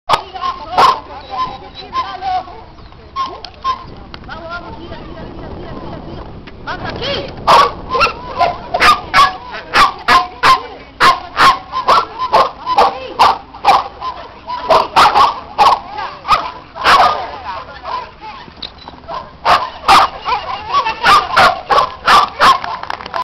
Wanda - Abierta de Agility 15/10/07
Wanda, una Border Collie y Nancy terminan la pista de la competencia abierta de Agility en Parque Roca.
Bien Wanda!! De a poco supera su terrpr por los bombazos!
saludos,
Nicole, Kiara & Kacey